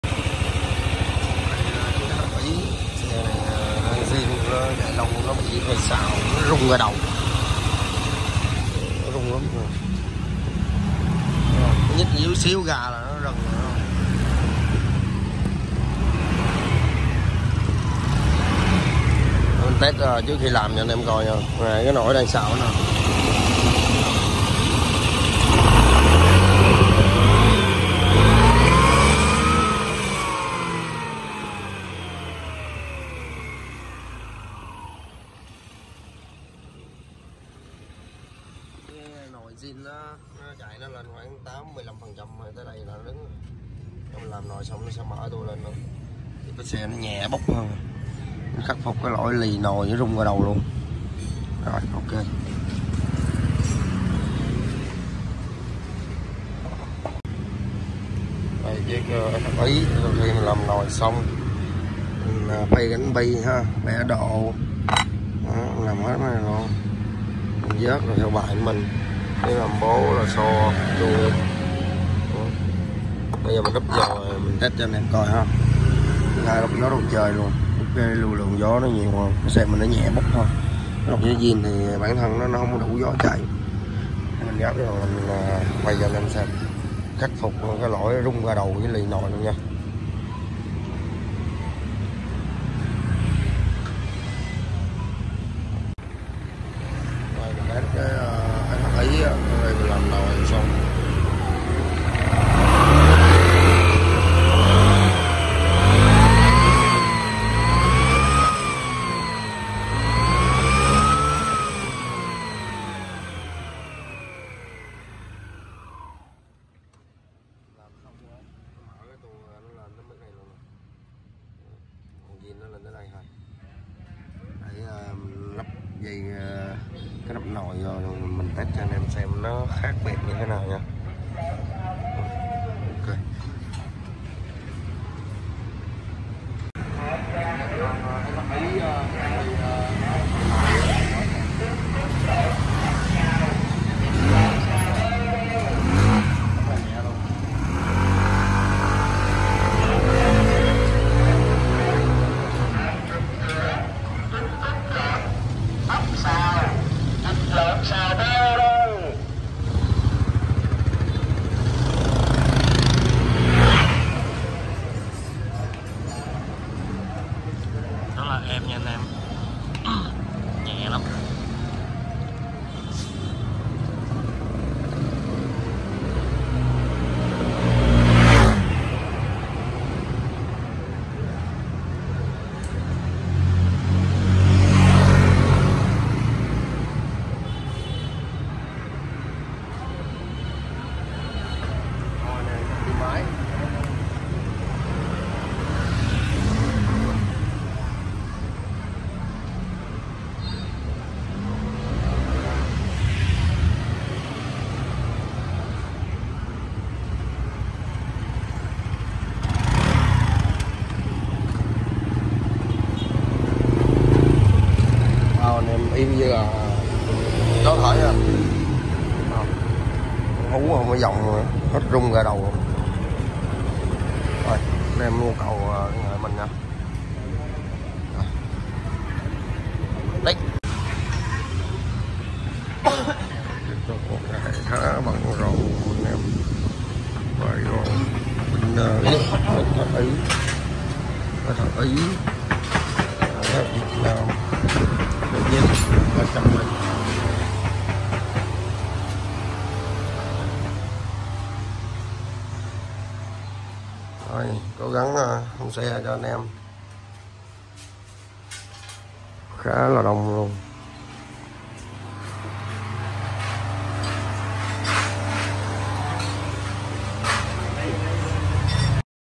Làm Nồi Đi nhẹ xe chạy bốc hơn xe zin rất nhiều cho Sh 150i Ý trị luôn bệnh rung ga đầu lì nồi luôn
Làm Nồi Đi nhẹ xe chạy bốc hơn xe zin rất nhiều cho Sh 150i Ý trị luôn bệnh rung ga đầu lì nồi luôn cho ae khách nha tại TBK Racing Team 147 Tân Khai P4 Q11 Bảo Hello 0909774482